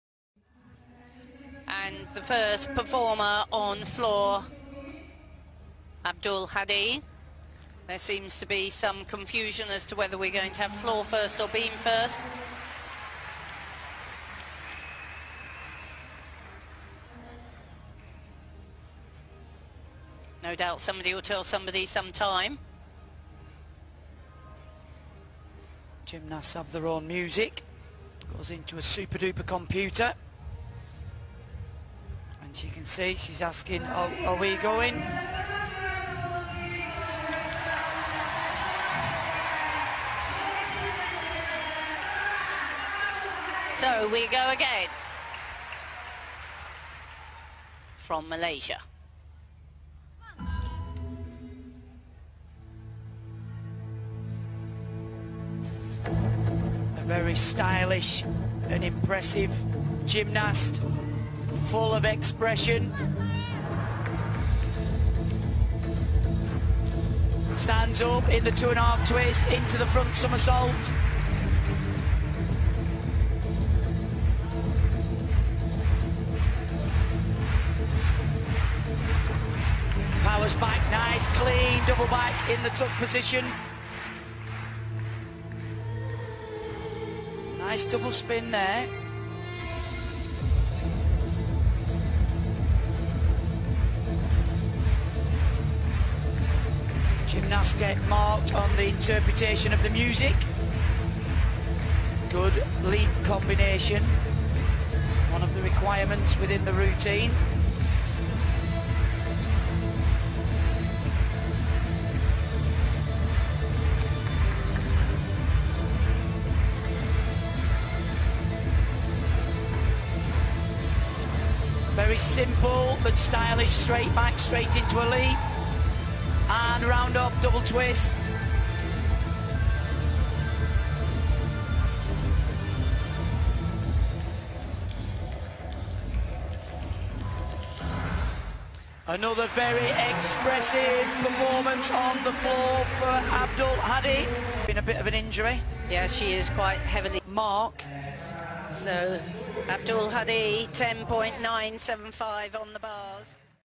【体操】ファラー・アン・アブドゥル・ハジ 2018年世界選手権 床運動
Farah Ann binti Abdul Hadi (born May 3, 1994) is a Malaysian artistic gymnast.
Farah was born in Subang Jaya, Selangor, to Abdul Hadi Ahmad and Canadian-born Kimberly Ann Gagnon on 3 May 1994. She is the second of three children and her sister, Katrina Ann, is a former national synchronized swimmer.
She took up gymnastics at age three and started to compete at national level competition Sukma Games. Farah Ann made her first appearance at the 2010 Games in New Delhi, scoring 12.050 points (floor exercise), 10.500 (beam) and 10.250 (uneven bars) as Malaysia finished fourth in the team event. In 2014, Farah Ann then took up the bronze in floor exercise and team event at Artistic Celtic Cup-Commonwealth Invitational in Perth, Scotland. Later that year, she finished in 11th place out of 24 gymnasts in the women’s individual all-round finals at the Commonwealth Games in Glasgow, Scotland. After the Commonwealth Games, she competed in the Asian Games in Incheon, South Korea. During the qualification stage, she placed sixth on the vaults (13.650 points), sixth in the uneven bars (12.800), 12th in the balance beam (12.250) and fourth in the floor exercise (13.050). She made it to the finals in the floor exercise, and finished 7th.
In 2015, she competed at the Southeast Asian Games (SEA Games), competing in all events — bars, beam, vaults, floor exercise, individual and team — in the preliminary round of the competition and made it to the finals of all events. She took up her first gold medal in the team's events, followed by a second in the floor exercise. She then gained a silver medal each in both the uneven bars, and individual events. The Malaysian gymnast won bronze medals in the vault, balance beam, as well as the uneven bars event, where another Malaysian gymnast Tan Ing Yueh took the gold.
After barely missing Rio 2016, she has qualified for the Tokyo 2020 Olympics fully by merit when she finished 59th out of 180 gymnasts in the qualifying session of the individual all-around event at the 2019 World Artistic Gymnastics Championships in Stuttgart, Germany. She is only the third Malaysian gymnast to qualify for the Olympics, after Au Li Yen in Sydney 2000 and Ng Shu Wai in Athens 2004.